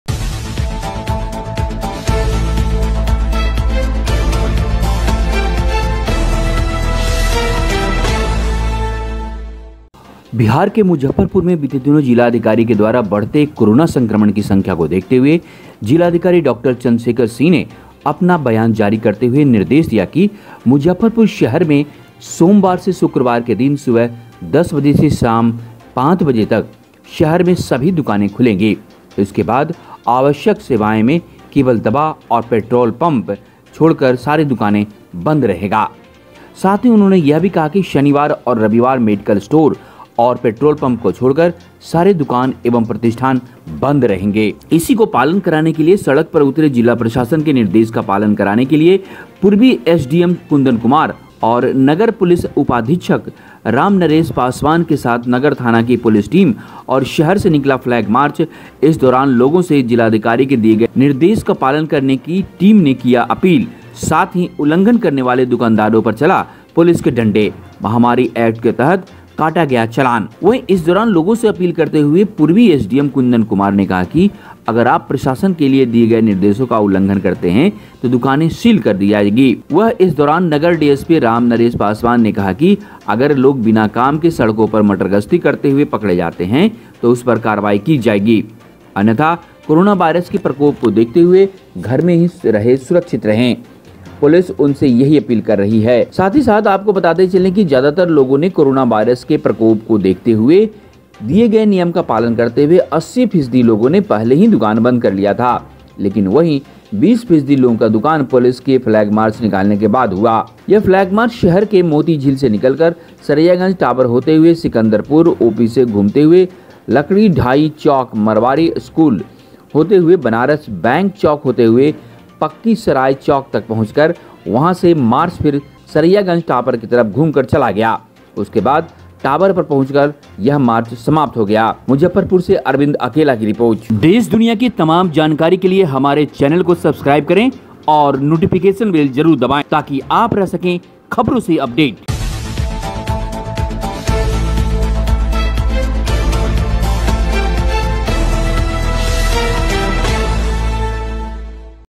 मुजफ्फरपुर शहर में दुकान बंद कराने को लेकर सड़क पर उतरी जिला प्रशासन की टीम | khabar bihar
मुजफ्फरपुर में बीते दिन जिला अधिकारी के के द्वारा बढ़ते कोरोना संक्रमण की संख्या को देखते हुए जिलाधिकारी डॉ चंद्रशेखर सिंह ने अपना बयान जारी करते हुए निर्देश दिया कि मुजफ्फरपुर शहर में सोमवार से शुक्रवार के दिन सुबह 10:00 बजे से शाम 5:00 बजे तक शहर में सभी दुकानें खुलेगी इसके बाद आवश्यक सेवा में केवल दवा और पेट्रोल पंप को छोड़कर सारी दुकाने बंद रहेगा